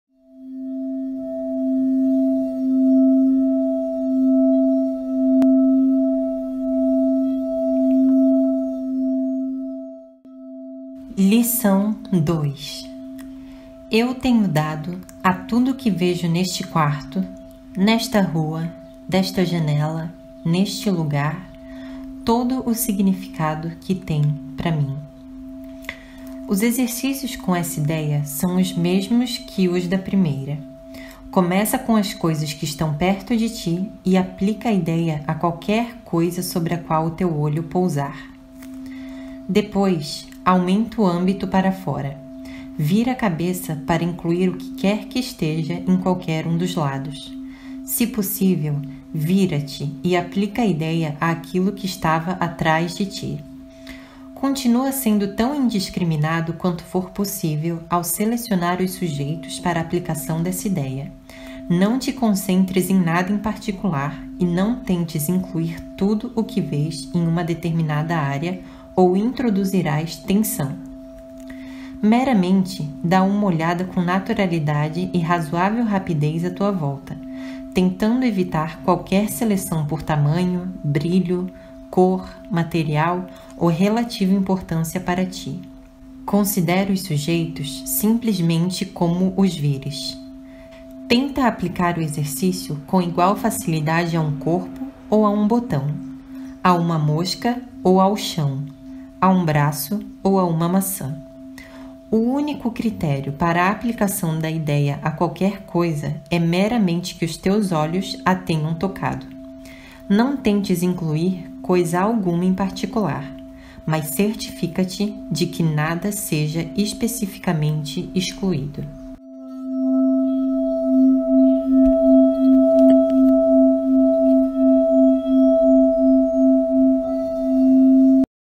Lição 2 | Um Curso Em Milagres
Lição 2 do livro de exercícios "Um curso em Milagres".

Eu tenho dado a tudo o que vejo neste quarto [nesta rua, desta janela, neste lugar] todo o significado que tem para mim.

1. Os exercícios com essa ideia são os mesmos que os da primeira. Começa com as coisas que estão perto de ti e aplica a ideia a qualquer coisa sobre a qual o teu olhar pousar. Depois, aumenta o âmbito para fora. Vira a cabeça para incluir o que quer que esteja em qualquer um dos lados. Se possível, vira-te e aplica a ideia àquilo que estava atrás de ti. Continua sendo tão indiscriminado quanto for possível ao selecionar os sujeitos para a aplicação dessa ideia, não te concentres em nada em particular e não tentes incluir tudo o que vês em uma determinada área, ou introduzirás tensão.

2. Meramente dá uma olhada com naturalidade e razoável rapidez à tua volta, tentando evitar qualquer seleção por tamanho, brilho, cor, material, ou relativa importância para ti. Considera os sujeitos simplesmente como os vires. Tenta aplicar o exercício com igual facilidade a um corpo ou a um botão, a uma mosca ou ao chão, a um braço ou a uma maçã. O único critério para a aplicação da ideia a qualquer coisa é meramente que os teus olhos a tenham tocado. Não tentes incluir coisa alguma em particular, mas certifica-te de que nada seja especificamente excluído.